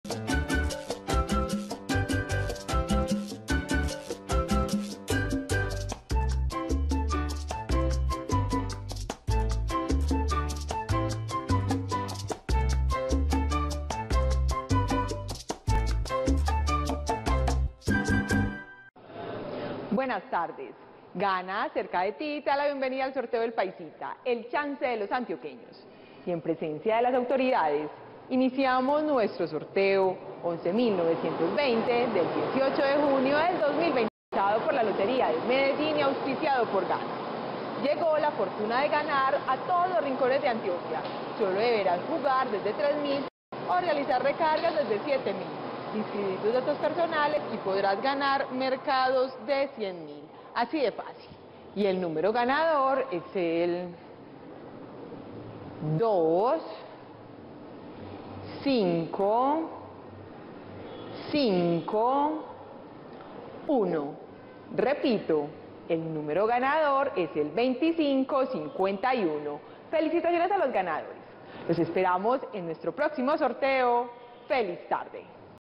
EL PAISITA 2 Resultados del Chance del SÁBADO 18 DE JUNIO de 2022 ✅🥇🔥💰
Hola buenas noches, te presentamos los resultados de las loterías y chances en Colombia, vamos con el Resultados del Chance El Paisita 2 del SÁBADO 18 DE JUNIO de 2022.